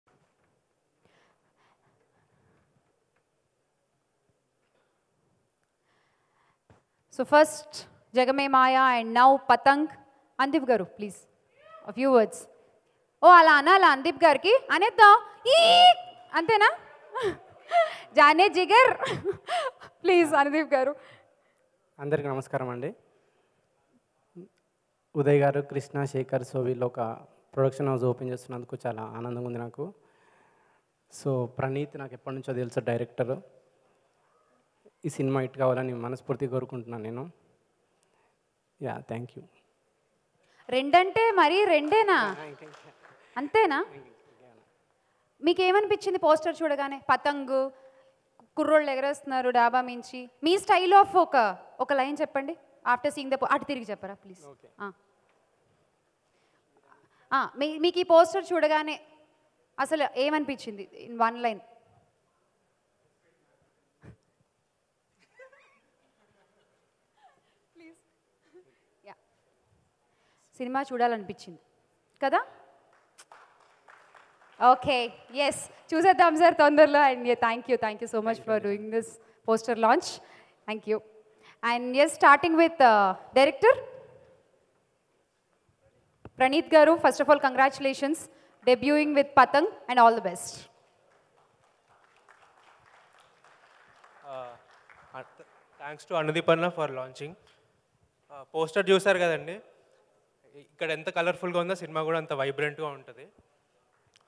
Anudeep Kv Hilarious Reply To Anchor Shyamala Question | Xappie Studios Launch Event | AL TV
WATCH HERE : Anudeep Kv Hilarious Reply To Anchor Shyamala Question | Xappie Studios Launch Event | AL TV